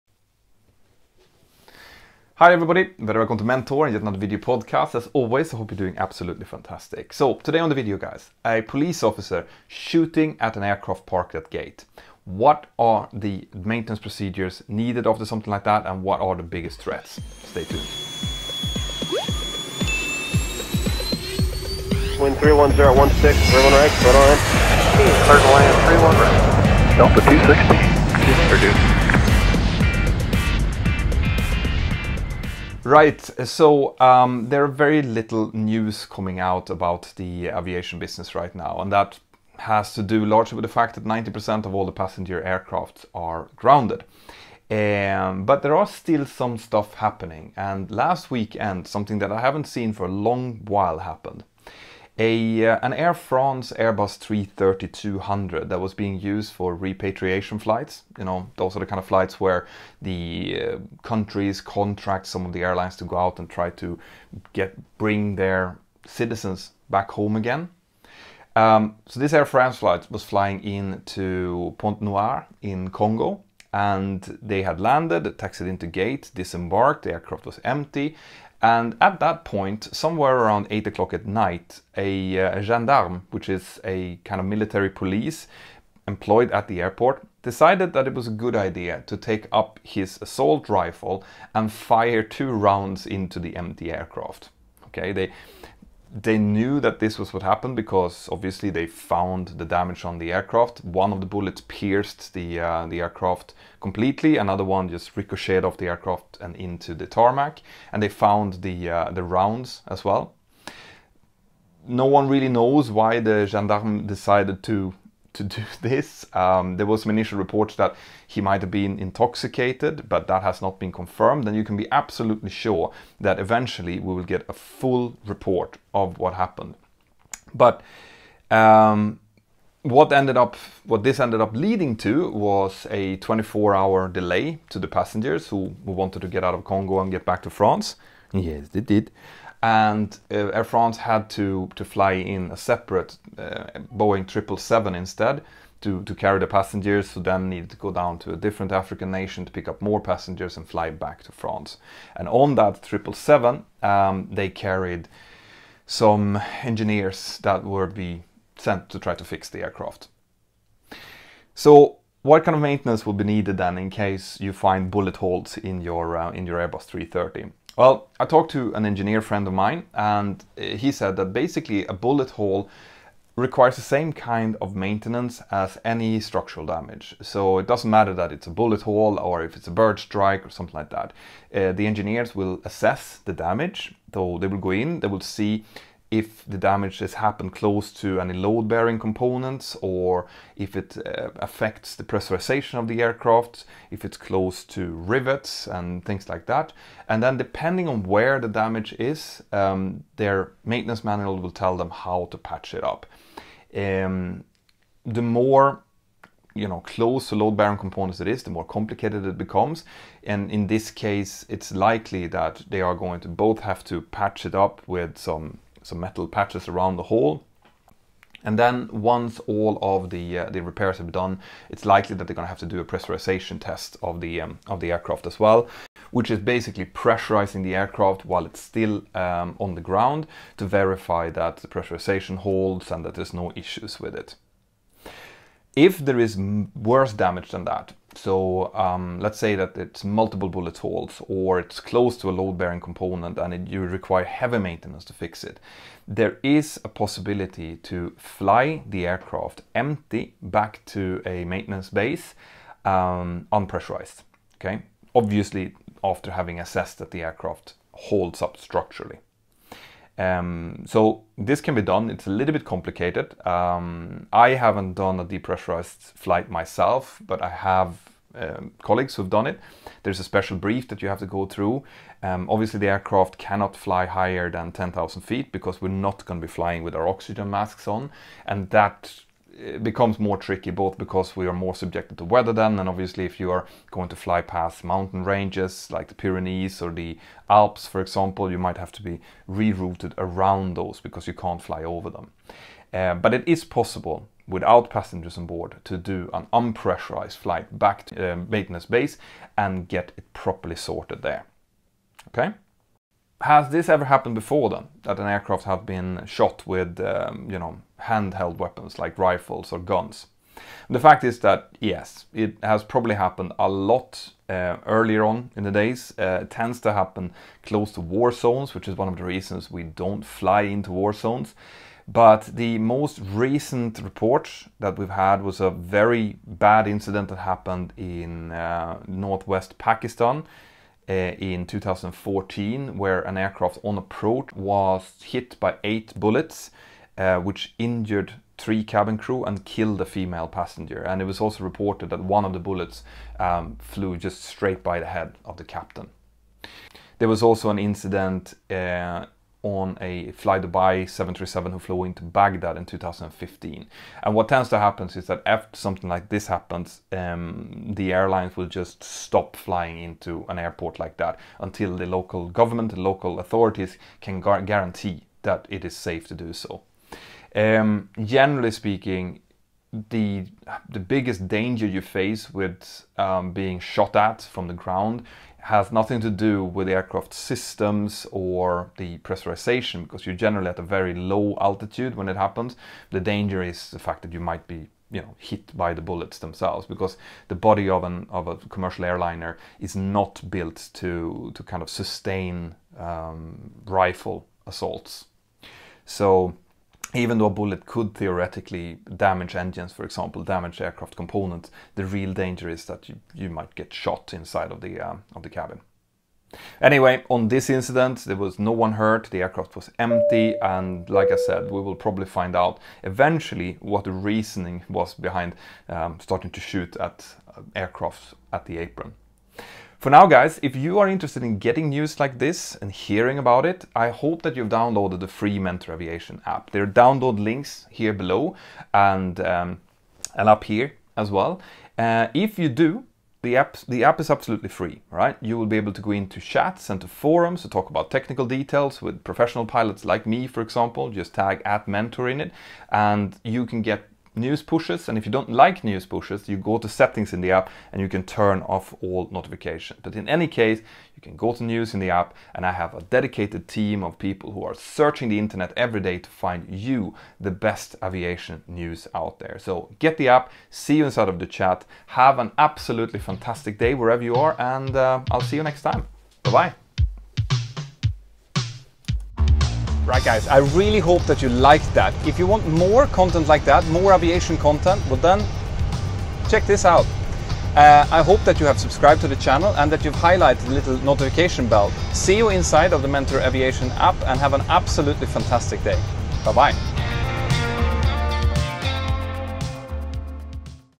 Airbus A330 shot by Police!
This Saturday, an Air France Airbus A330-200 was pierced by bullets from a Gendarmerie PMAK Assault rifle when it was parked at gate in Point Noire, Congo. One of the bullets pierced the fuselage and the other just graced the aircraft and ended up in the tarmac.
The shooter was identified as an, on duty, gendarmerie officer and the motive for the shooting is still unclear.
In this video I will talk about the engineering response to an event like this as well as other similar events and the risks associated.

A huge THANK YOU to the featured channels in todays video.